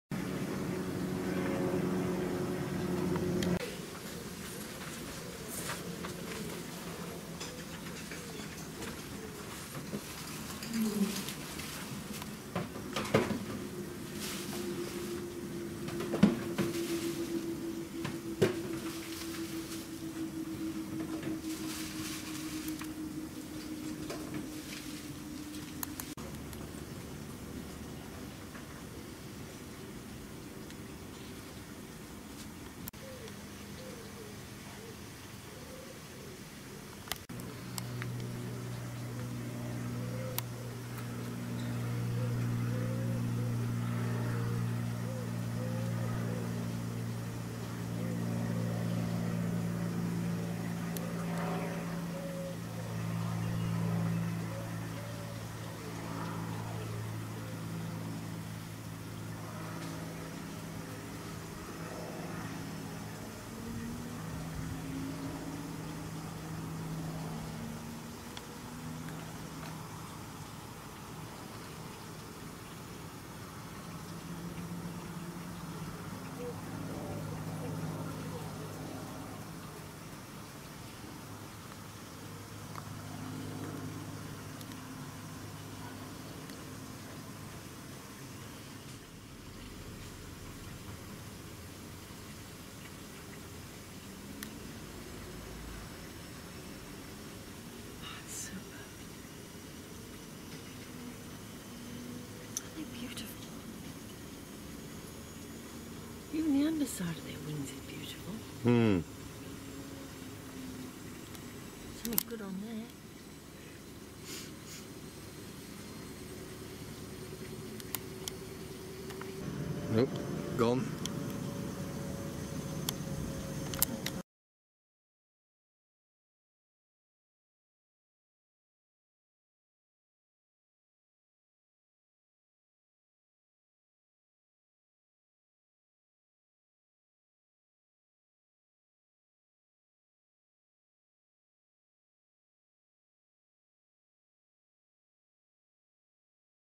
Releasing the Painted Lady Butterflies.
Release of the Painted Lady butterflies bred in captivity for returning to the wild as adults. A couple seemed reluctant to go!